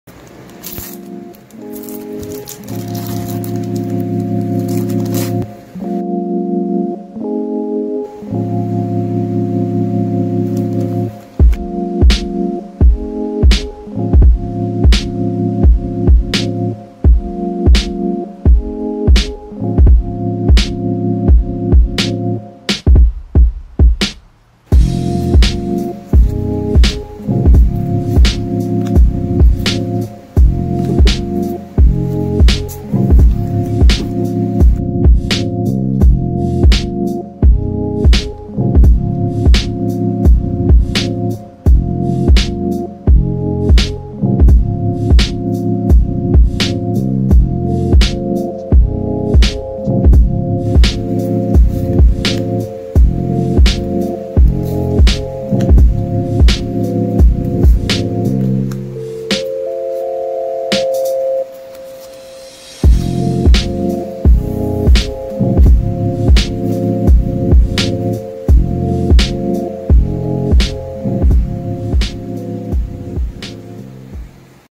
S02E23: NCOV Scare... Always wash your feathers... Open booster packs!
Magic: The Gathering Booster Pack Openings from the Philippines! War of the Spark! You can also buy MTG card singles on our Facebook